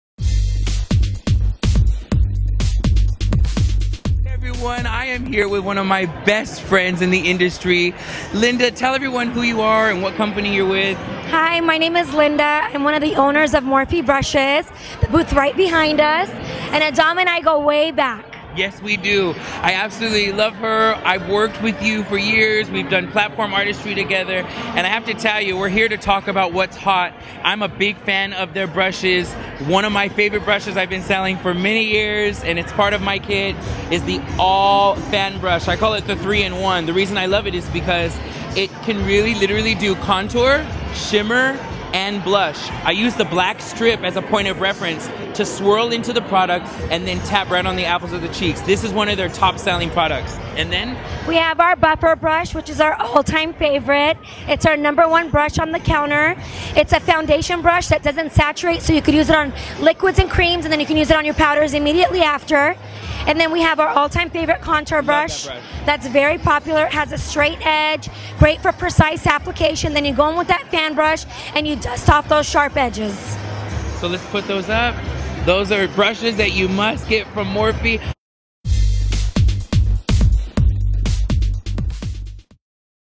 Morphe's Top-3 Selling Brushes
Adamme speaks with Linda Tawil, co-owner of Morphe Brushes, who identifies their top-3 selling brushes - their fan, contour, and buffer brushes. Filmed at the 2014 PHAMExpo in Pasadena California.